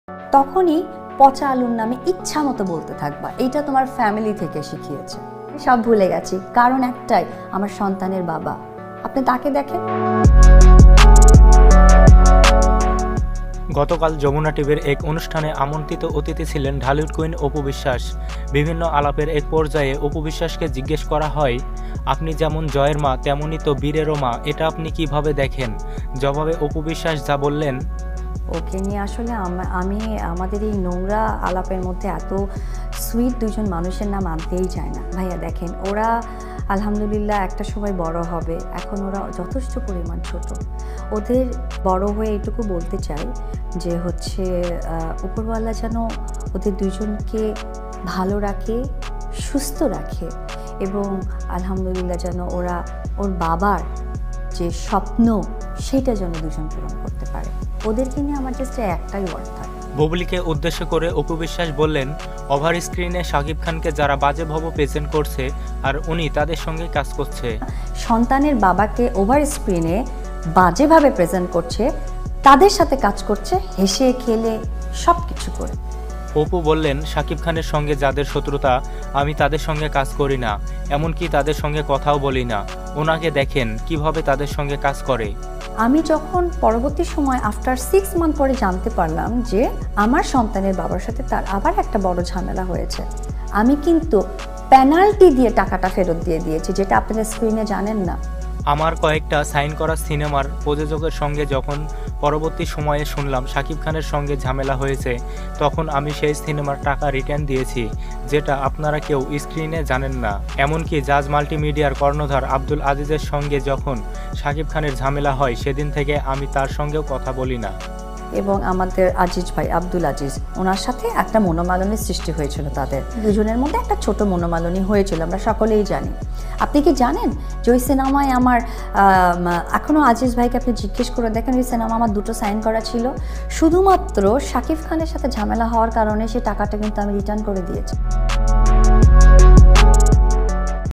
পঁচা আলুর সন্তানের বাবা কে | তোমার শত্রুর সঙ্গে কাজ করছে টুবলি | Apu Biswas | Shakib Khan | Bubly
পঁচা আলুর সন্তানের বাবা কে | তোমার  শত্রুর সঙ্গে কাজ করছে টুবলি | Apu Biswas | Shakib Khan | Bubly | joy | Bir
গতকাল যমুনা টিভির এক অনুষ্ঠানে আমন্ত্রিত অতিথি ছিলেন ঢালিউড কুইন  অপু বিশ্বাস বিভিন্ন আলাপের এক পর্যায়ে অপু বিশ্বাস কে জিজ্ঞেস করা হয়।

আপনি যেমন জয়ের মা তেমনি তো বীরেরোমা এটা আপনি কিভাবে দেখেন।
জবাবে অপু বিশ্বাস যা বললেন।